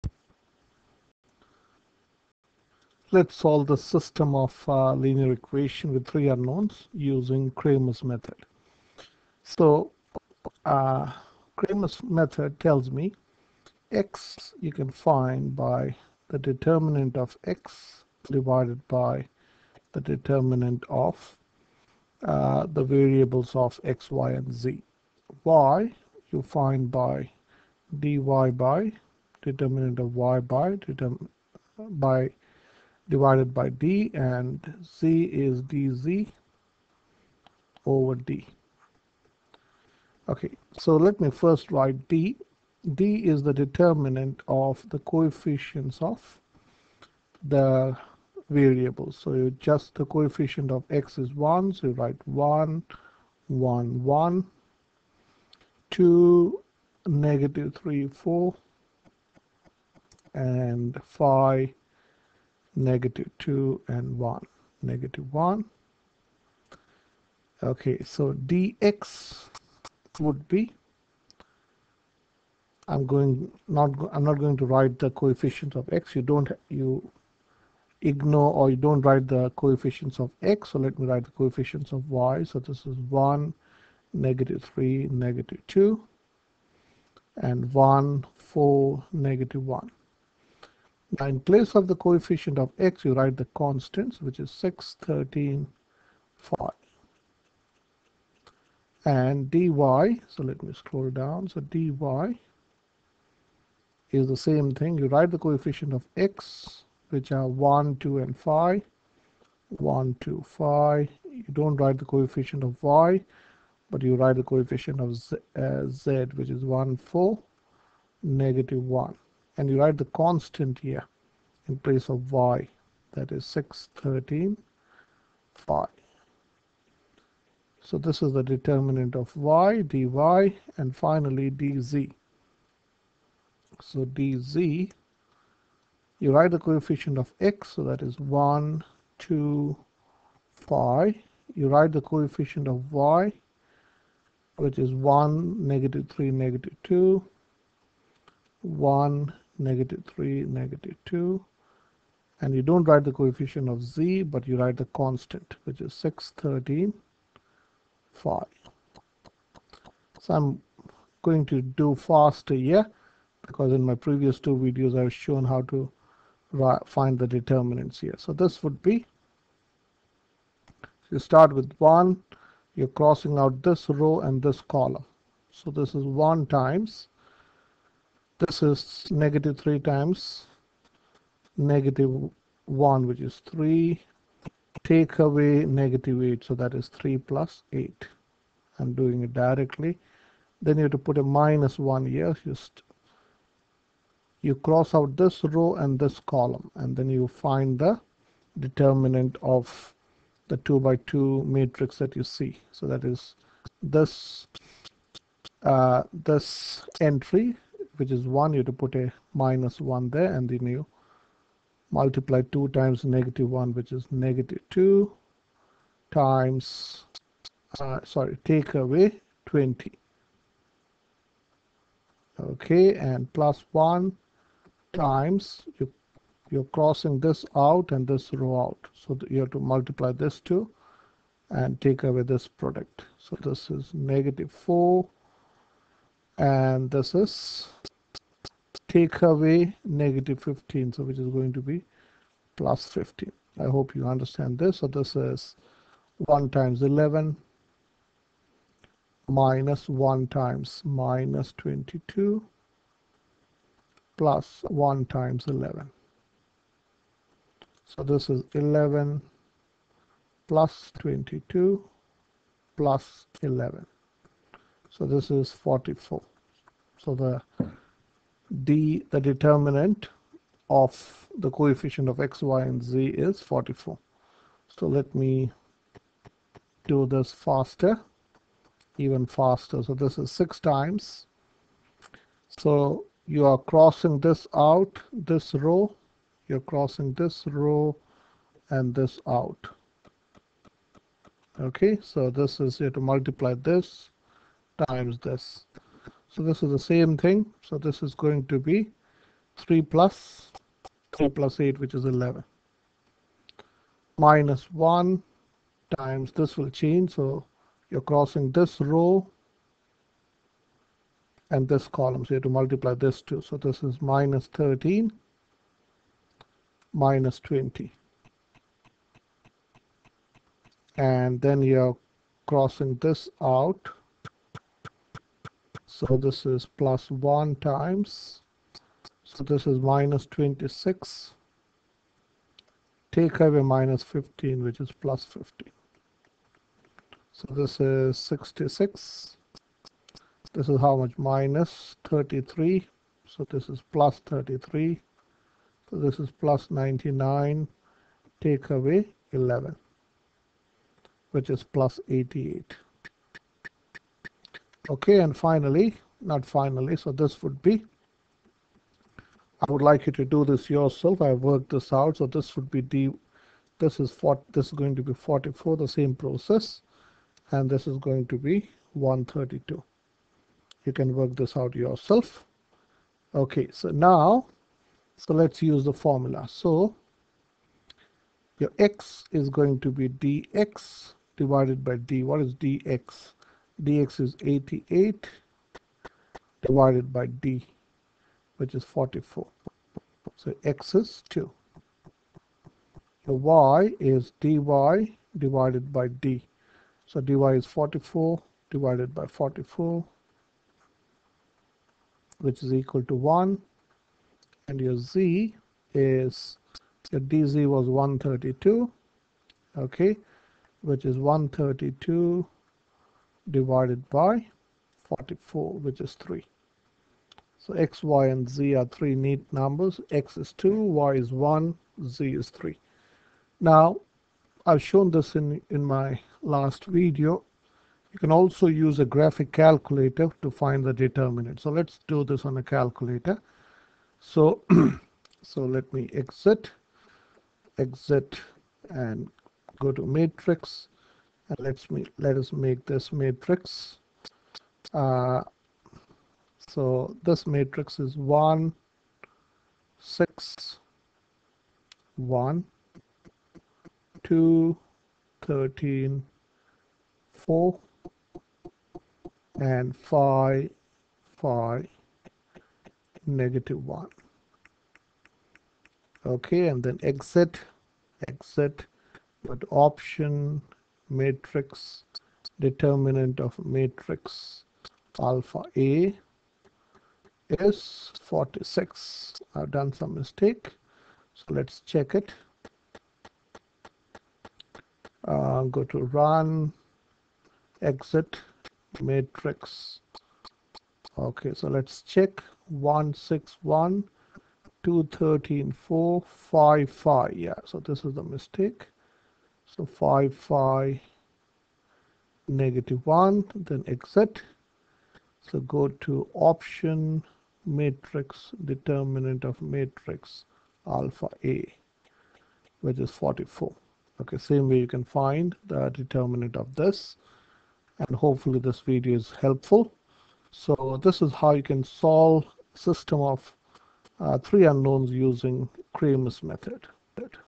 Solving simultaneous equation with three unknown using Cramer's Rule
In this video I have used Cramer's Rule to solve simultaneous equation with three unknowns